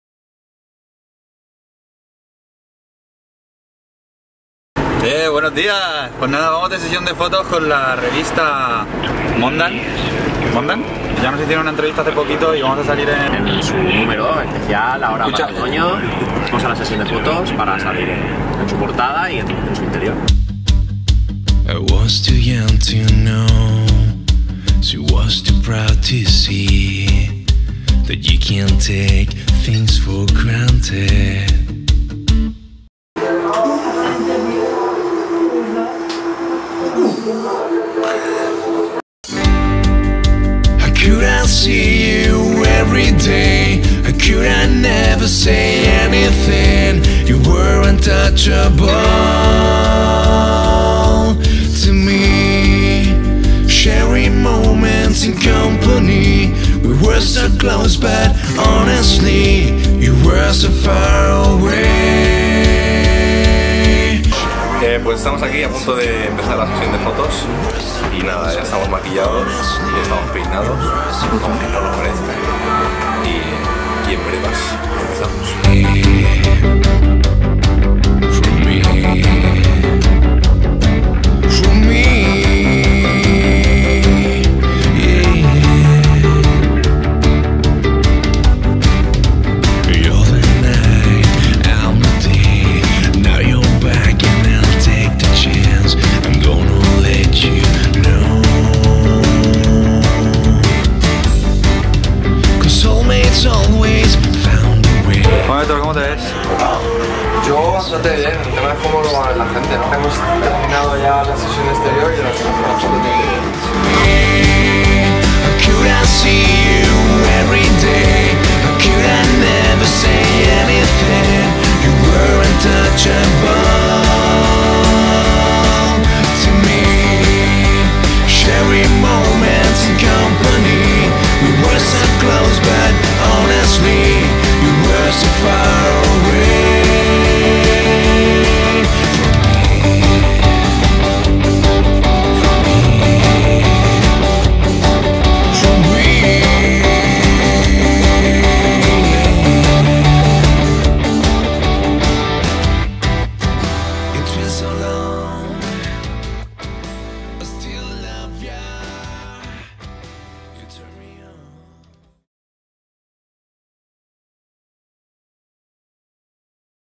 Making Of: Sesión de fotos para Möndan (Especial Otoño 2013)
LoveMeBack

Fotografía: Paula Guilló
Maquillaje y estilismo: Elena Graciá
Vestuario: Compañía Aérea
Escenografía: Casas Naturales